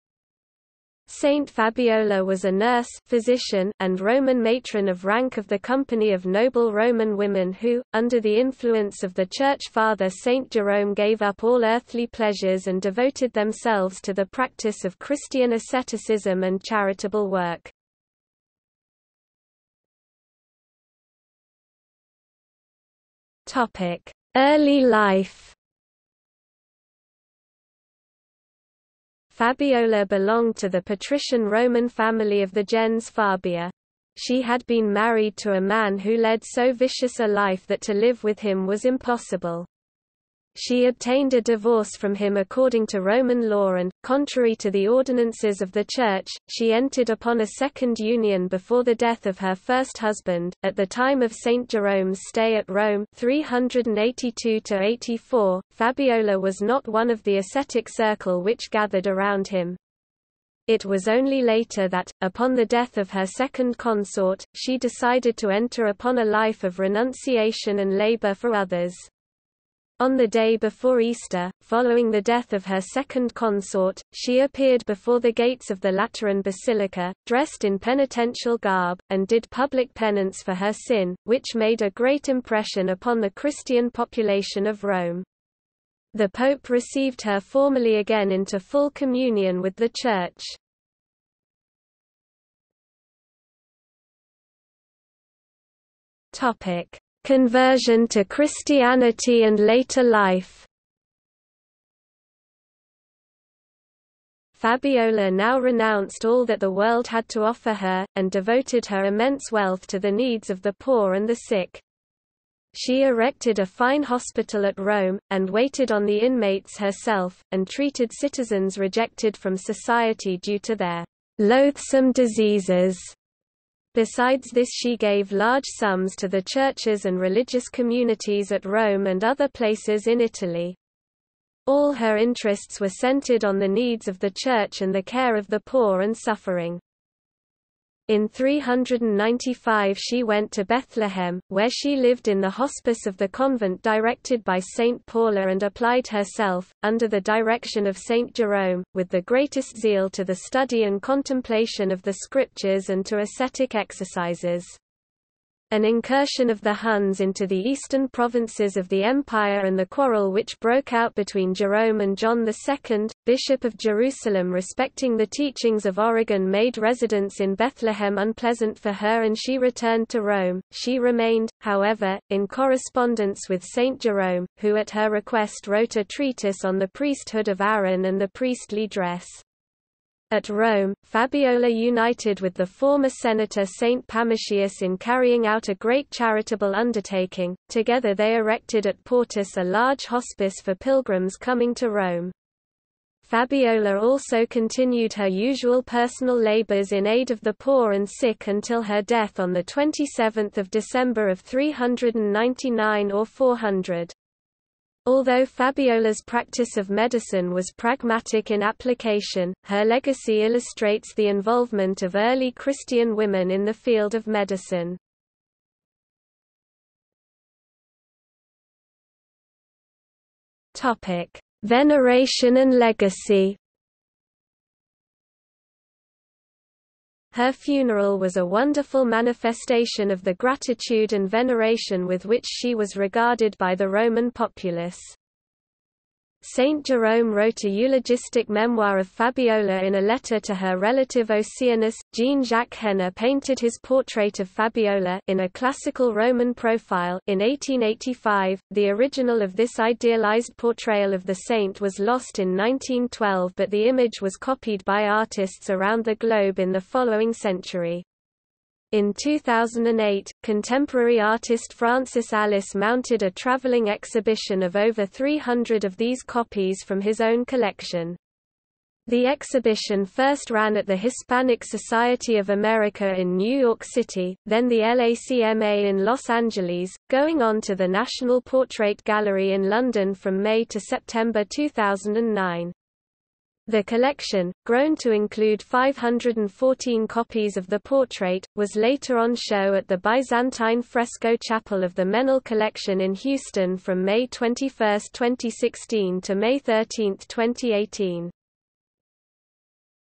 Saint Fabiola | Wikipedia audio article
00:00:19 1 Early life
00:01:32 2 Conversion to Christianity and later life
00:03:37 3 Veneration and Legacy

- Socrates

SUMMARY
Saint Fabiola was a nurse (physician) and Roman matron of rank of the company of noble Roman women who, under the influence of the Church father St. Jerome gave up all earthly pleasures and devoted themselves to the practice of Christian asceticism and charitable work.